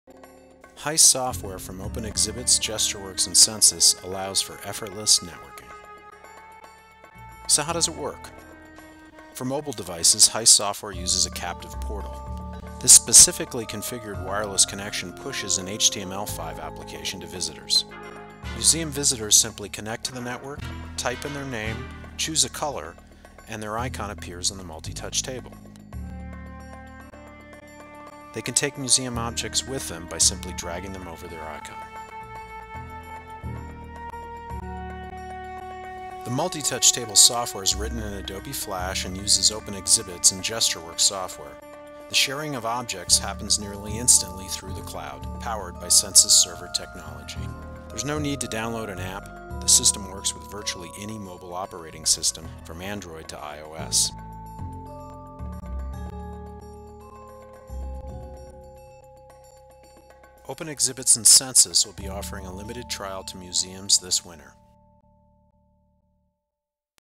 Heist
Heist is an experimental project that uses Open Exhibits and GestureWorks software, powered by Sensus server technology.  A test-bed involving 10 museums will begin in December 2011.

If your museum is interested in participating please visit us